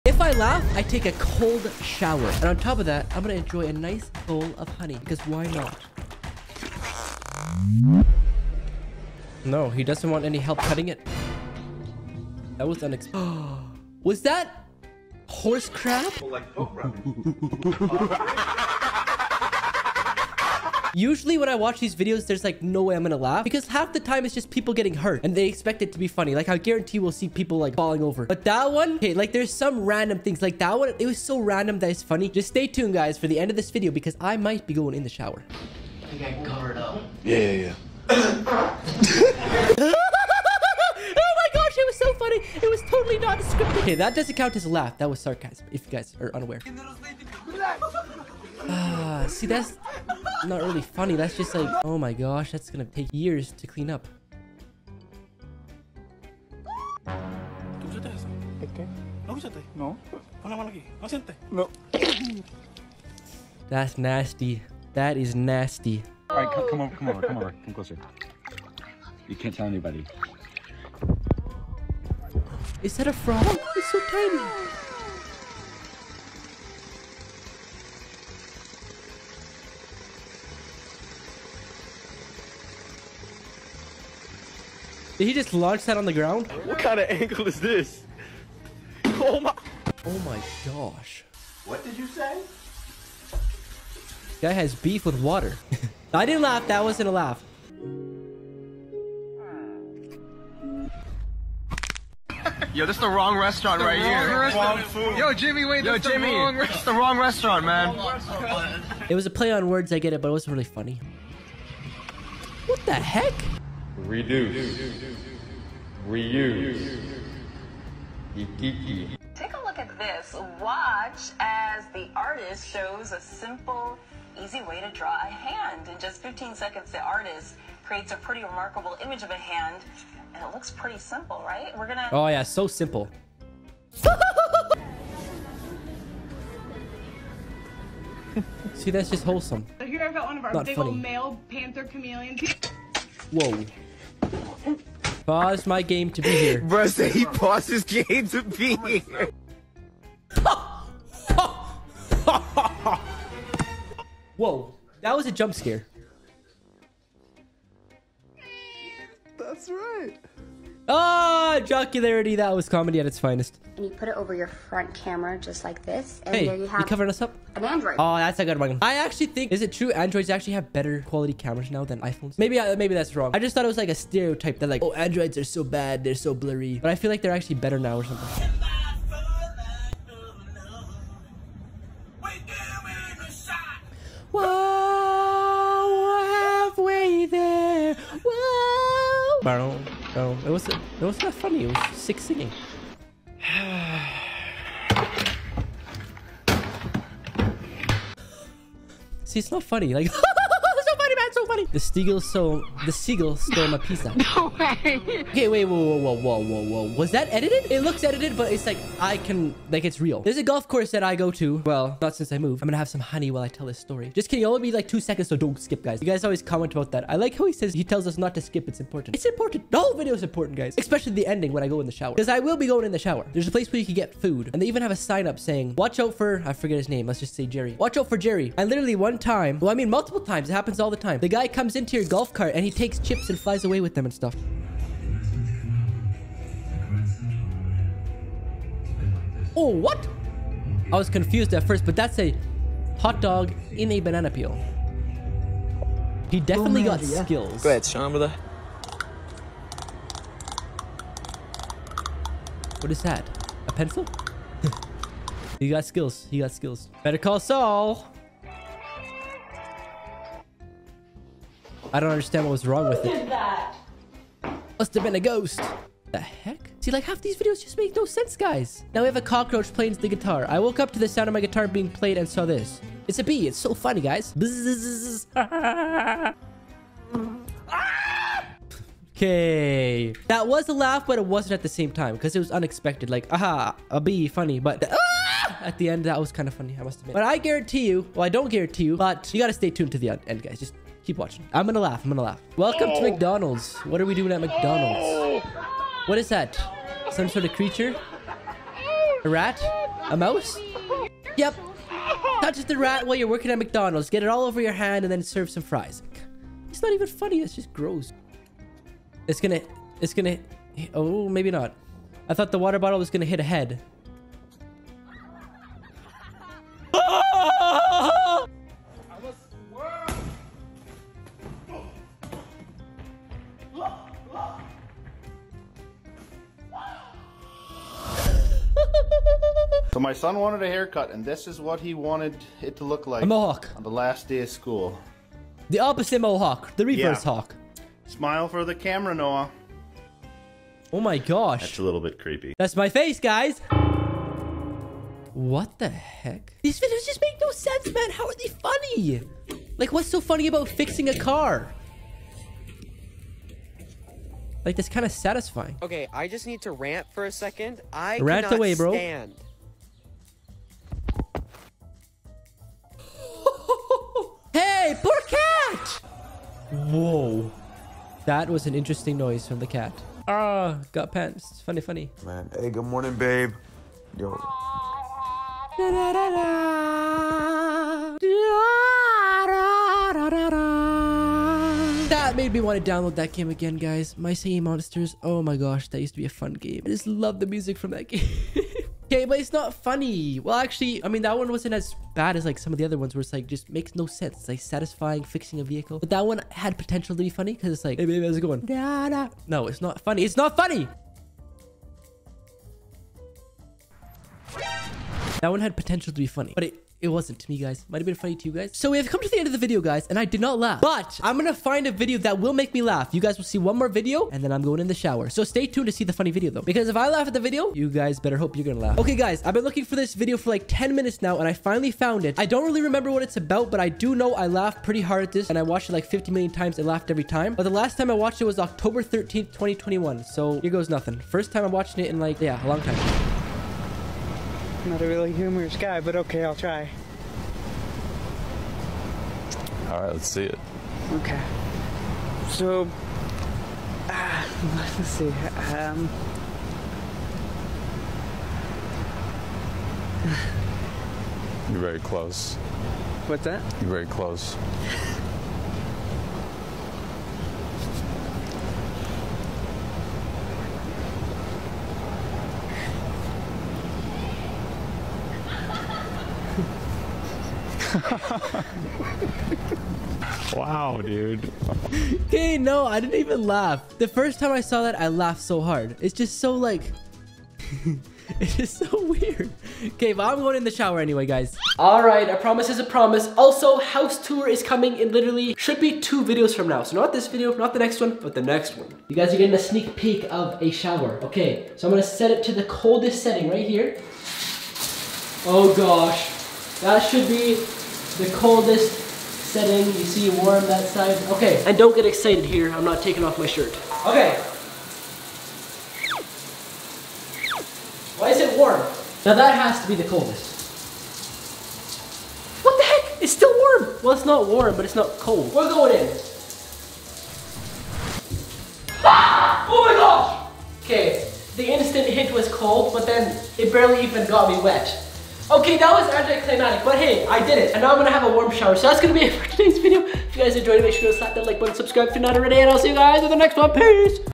If I Laugh, I Take A Cold Shower
In todays video I react to funny videos and if they make me laugh I take a cold shower

My Gear:
Mouse Pad